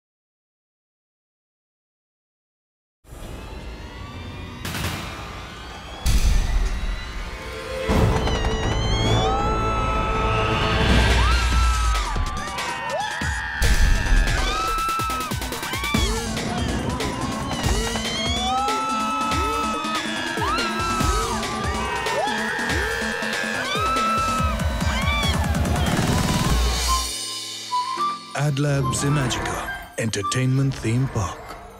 Gold Rush Express at Imagicaa!
This coaster will take you on an American West tour. From the ravines and the ranch grounds of the cowboys to the parlors and the saloons of the pistol wielding outlaws and sheriffs, experience it all at the Gold Rush Express!
Scream, shout and Sleigh high in this roller coaster!